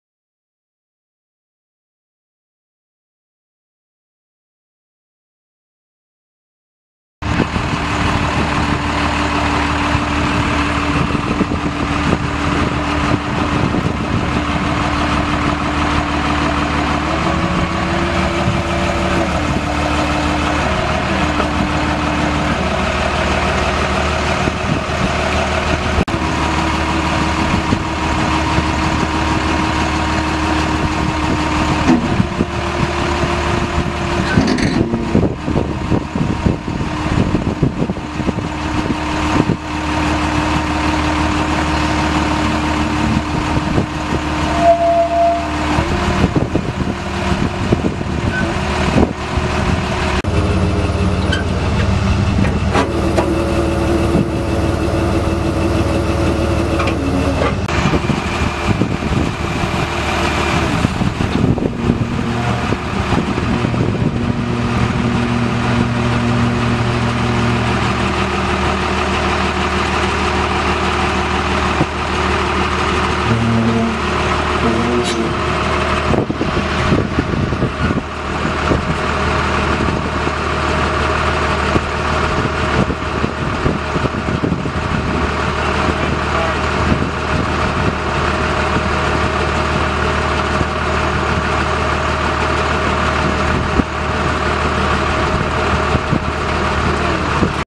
1992 Landoll 300B equipment trailer for sale | sold at auction September 10, 2015
1992 Landoll 300B equipment trailer
29&#039;L x 8&#039;W
Braden winch
20,000 lbs capacity
Spring suspension
Air brakes
Tandem axle
750R16 14ply tires
GVWR 29,000 lbs
GAWR 10,000 lbs per axle
1LH300HH3N1006155

It was located in Fairview, KS, and we sold it to the highest bidder for $7,370.